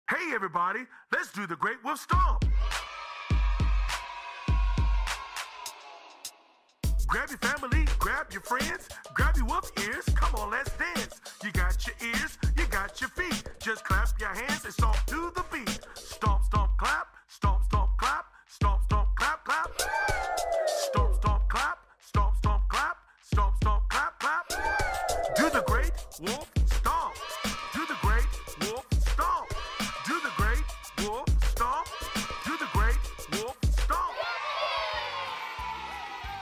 Let’s Learn the Great Wolf Stomp!
It’s everybody’s favorite Great Wolf Lodge dance: the Great Wolf Stomp! Learn all the moves with us for National Dance Day and let us hear your loudest hoooooowl!